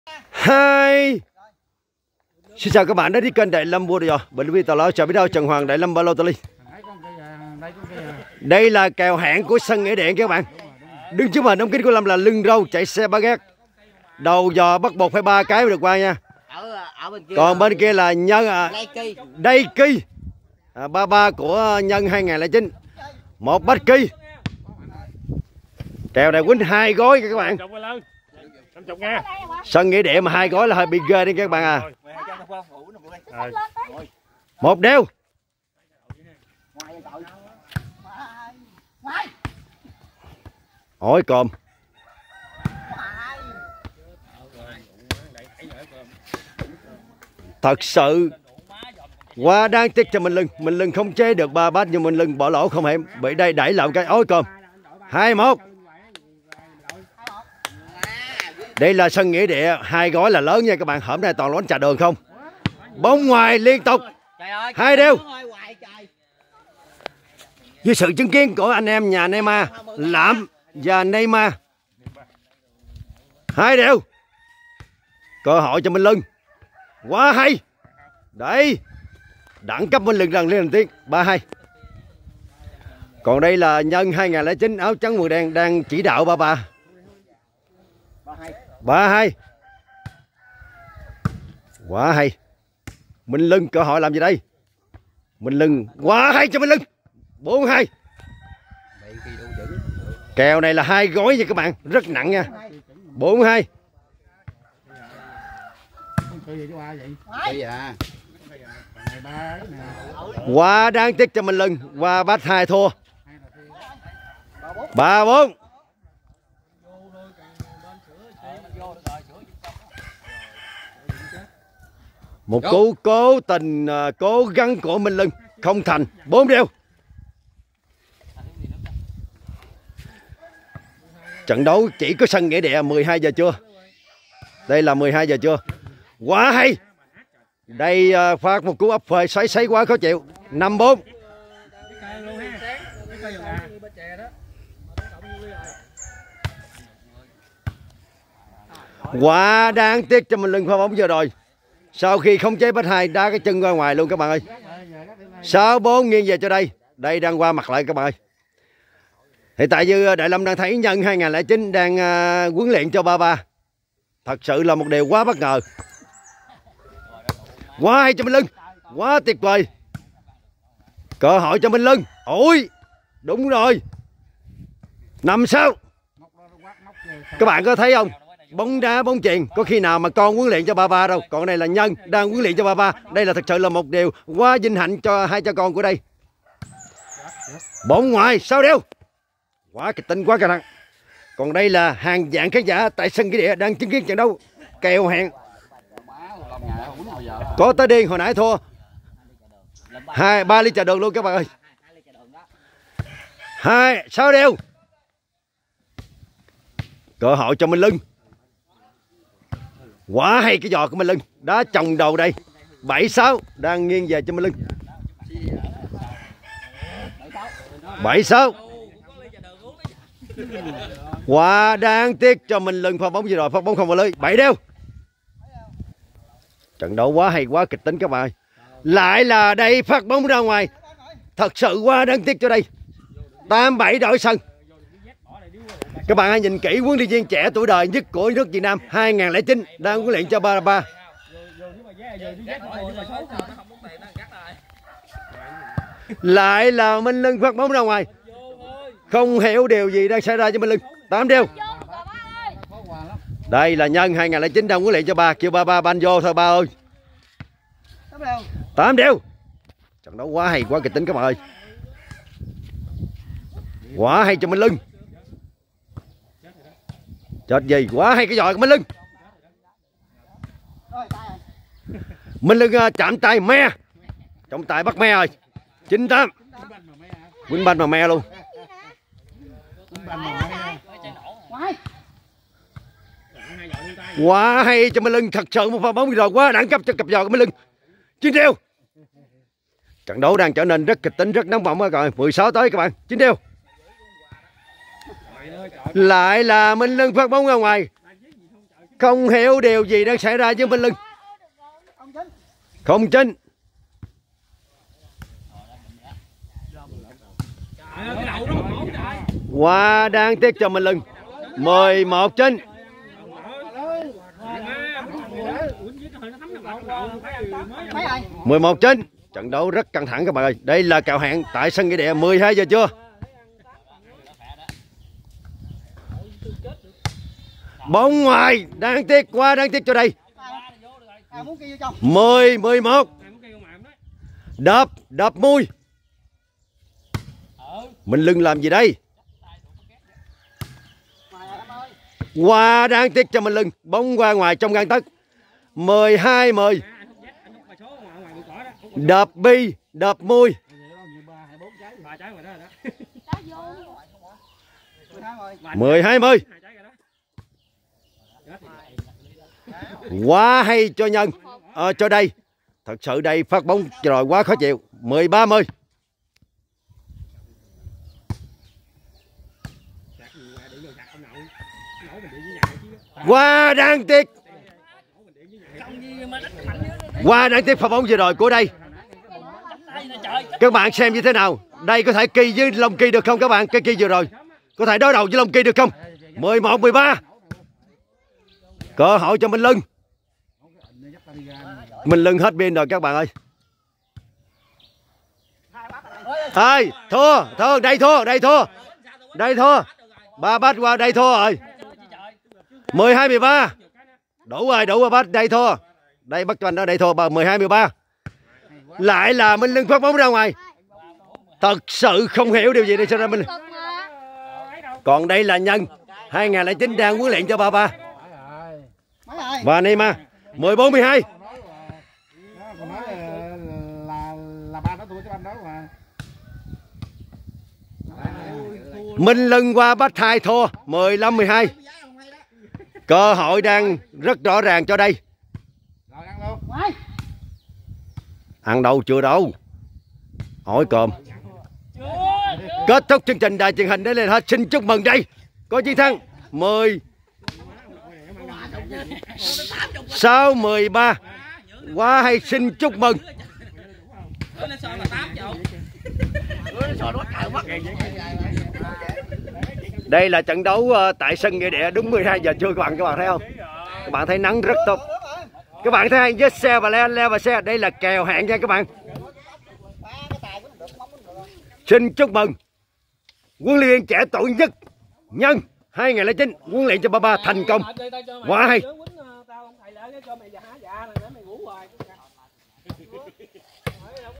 Kèo hẹn Luân râu đầu giò buộc giữ 3 bas *Đây ki* đánh tay tự do 2 gói sân Nghĩa địa sét 2
Sân nghĩa địa đang căng thẳng leo thang của các cao thủ làng quê...
Mo Mo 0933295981 trần hoàng đại lâm.